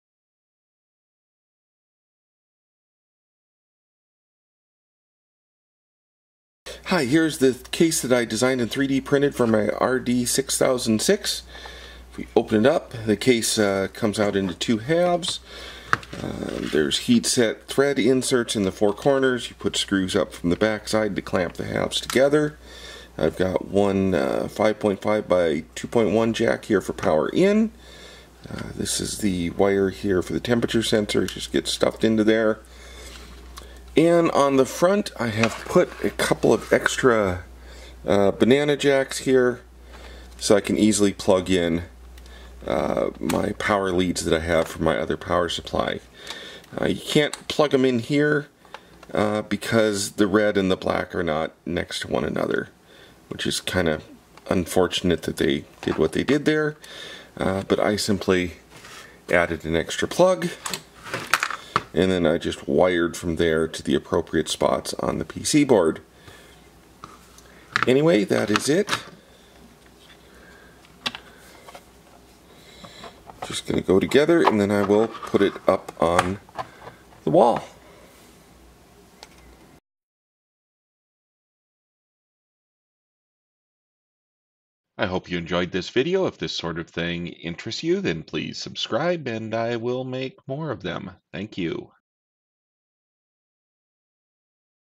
3D Prints 23: RD6006 Power Supply Wall Mount Case
This is a 3D Printed case for the Riden RD6006 power supply. The case prints as two pieces which are secured by M2.5 machine screws and M2.5 heat-set thread inserts. The two additional banana jacks are hardwired to the red and black binding posts on the RD6006 power supply board.